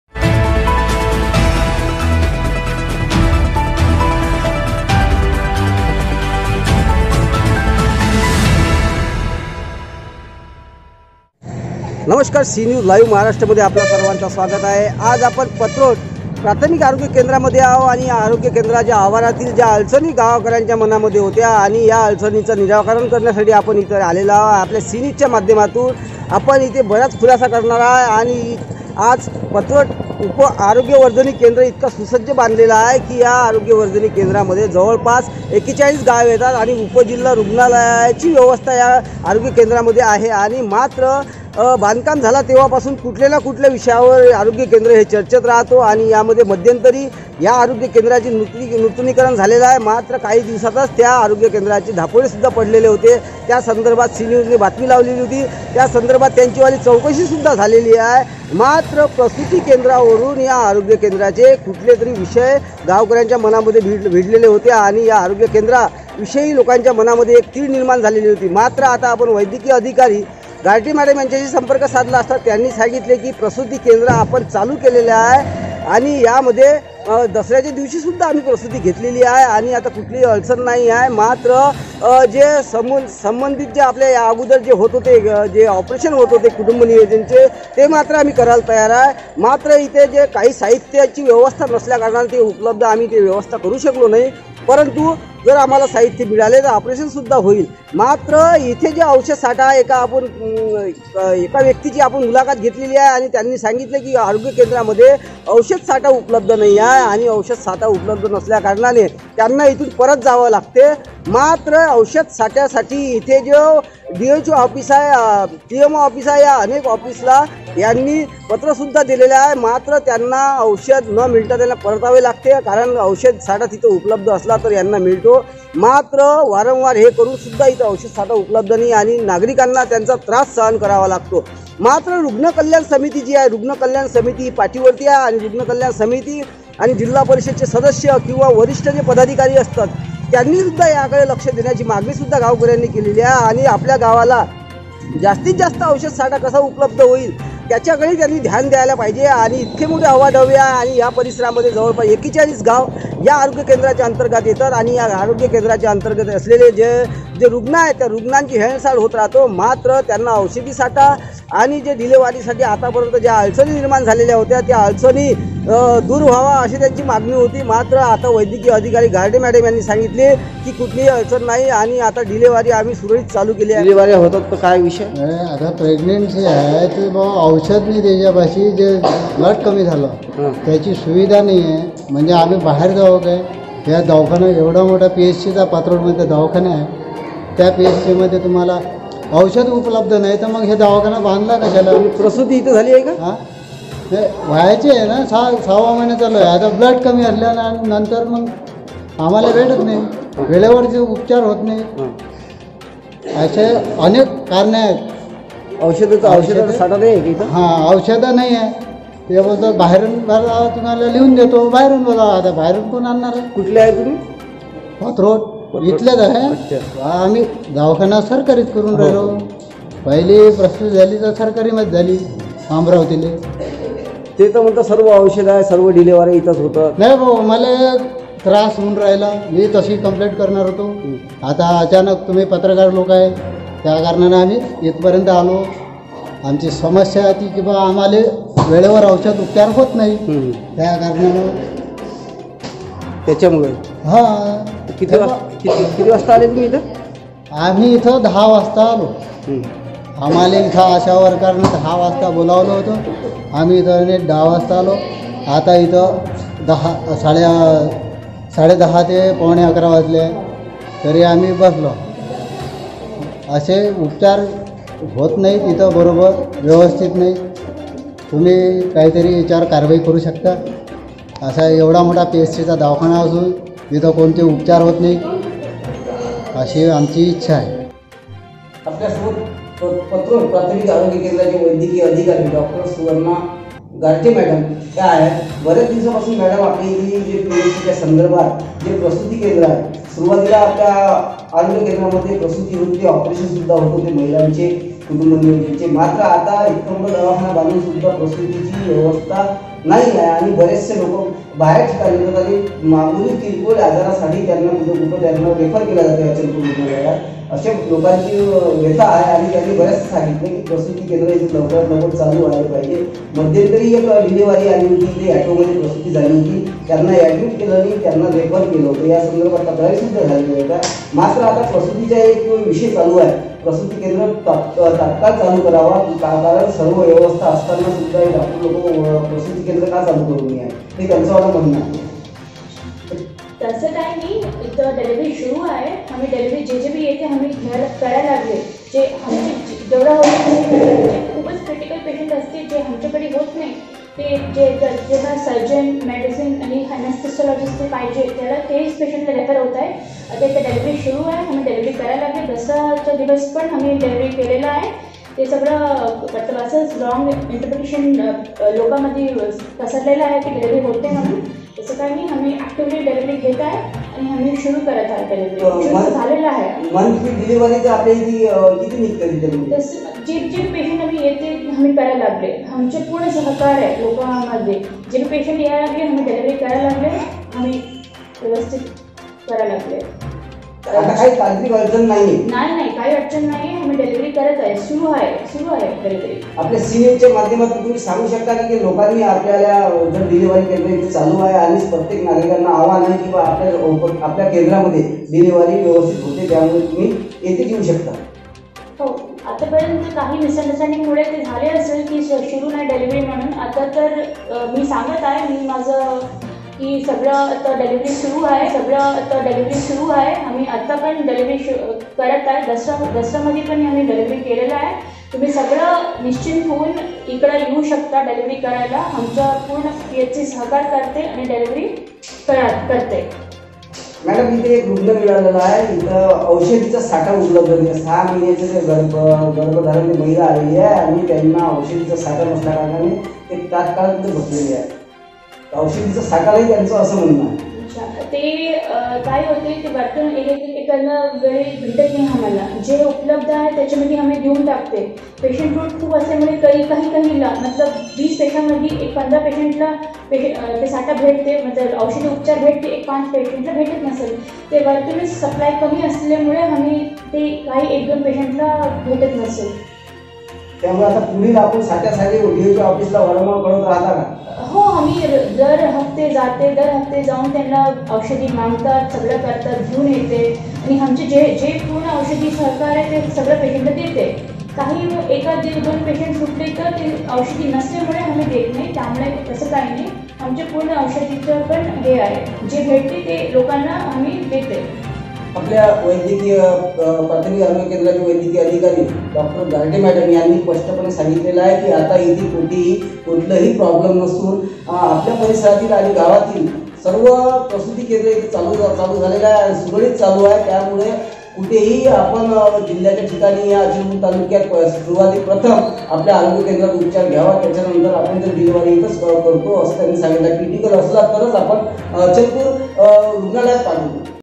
पथरोट प्राथमिक आरोग्य केंद्र वैध्यकीय अधिकारी डॉक्टर सुवर्णा गारते यांची प्रसूती सन्दर्भात
पथरोट प्राथमिक आरोग्य केंद्र वैध्यकीय अधिकारी डॉक्टर सुवर्णा गारते यांची प्रसूती सन्दर्भात प्रतिक्रियां,
मात्र रुग्णला औषद् उपलब्द नसल्याची तक्रार,
बघा रुपेश फरकुंडे ची ग्राउँड रिपोर्ट ।